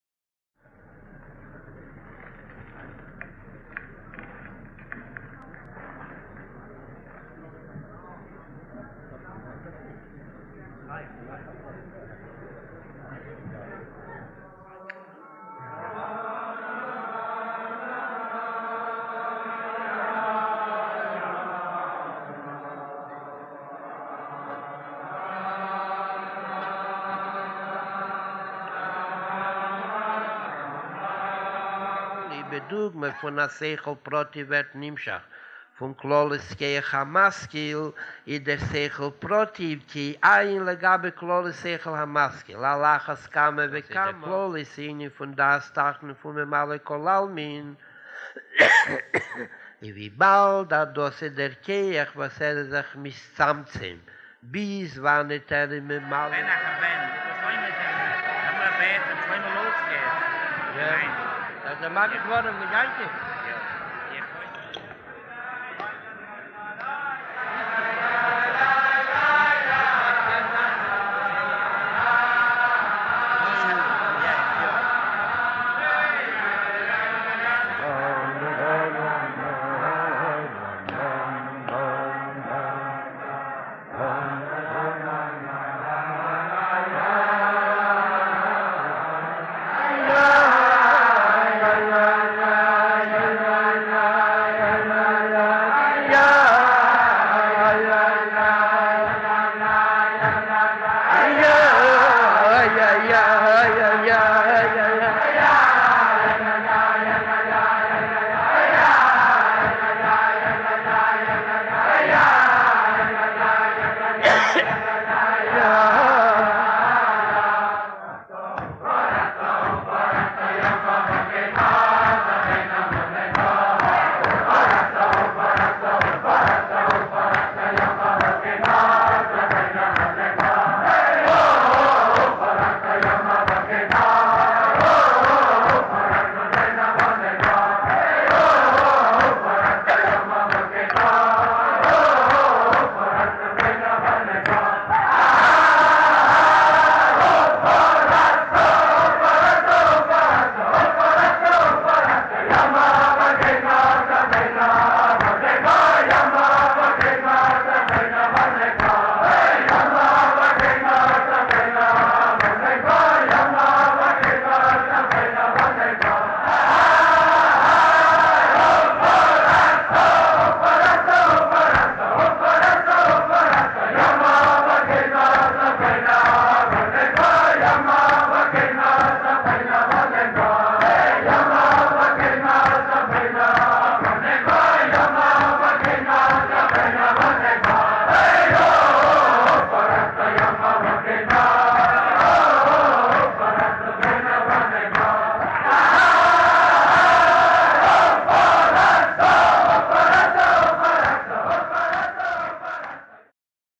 Farbrenguen Purím 5723
Atisbos únicas de un farbrenguen Purim de una película muda recién adquirida, con audio complementaria del mismo evento.

Purím, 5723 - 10 de Marzo, 1963